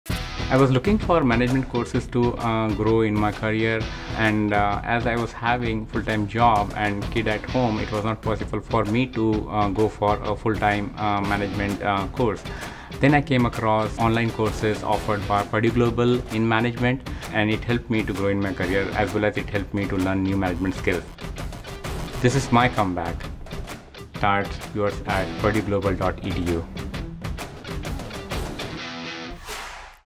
This is my comeback | Tushar, Management ’19
Tushar wanted to advance into a leadership role at work, but between being a dad and working full time, he knew a traditional degree program wouldn’t work for him.

That’s why Purdue Global’s program in project management was his opportunity for a comeback – it gave him the flexibility he needed to focus on what was next for his family and his career.

Backed by PurdueUniversity, one of the nation’s most respected and innovative public universities, Purdue Global was built specifically for working adults like Tushar who bring their life experience into the online classroom.

Start your comeback at purdueglobal.edu.